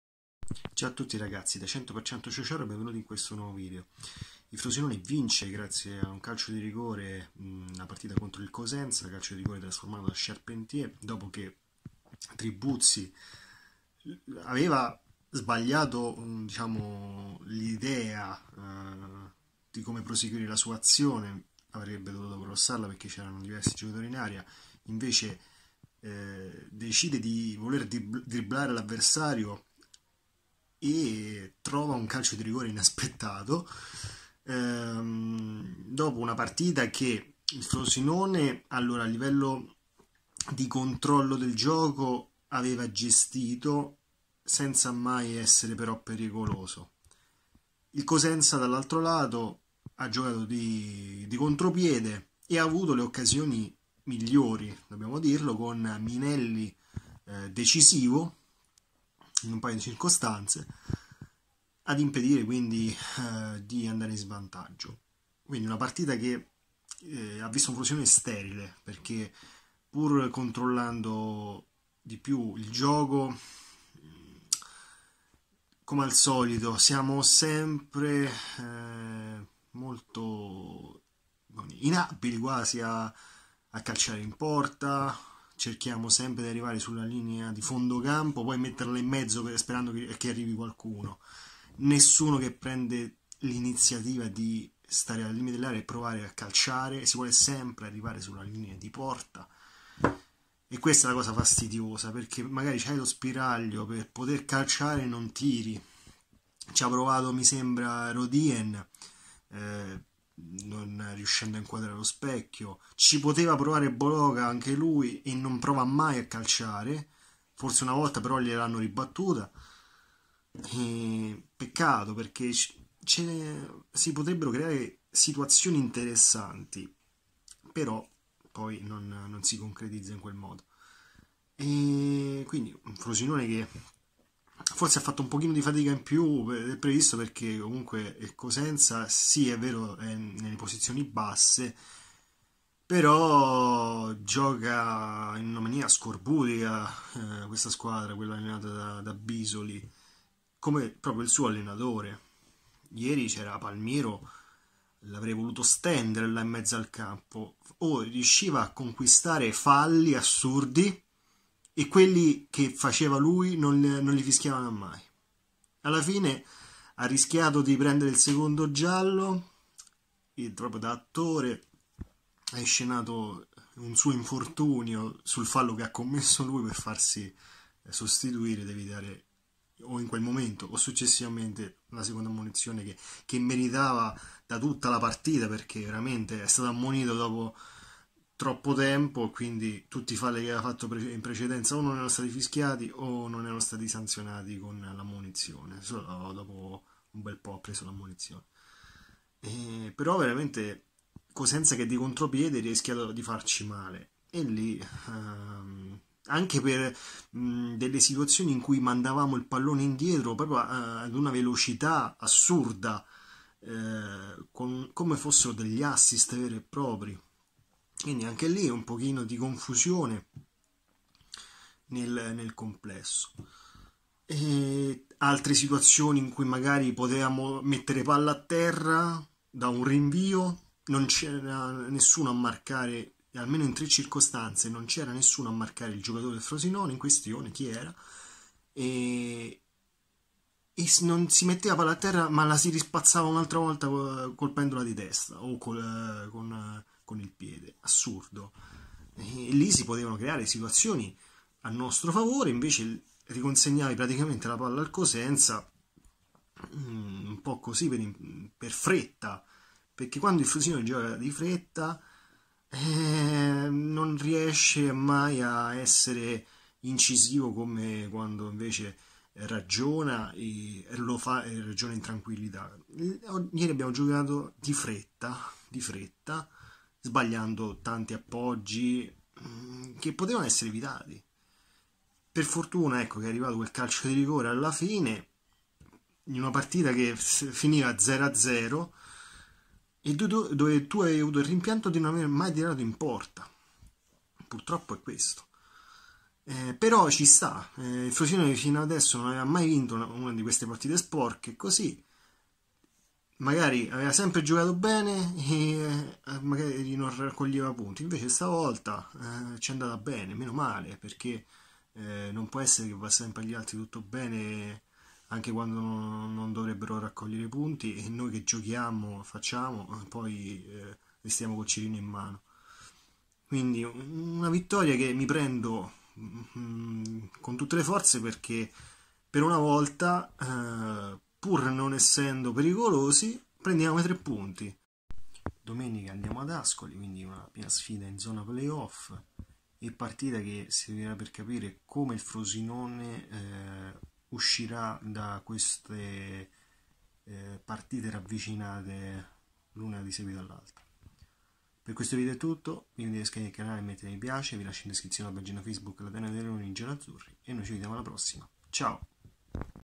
Frosinone - Cosenza 1-0: una dolce vittoria sporca
Un rigore procurato da Tribuzzi e trasformato da Charpentier permette al Frosinone di vincere nonostante le difficoltà.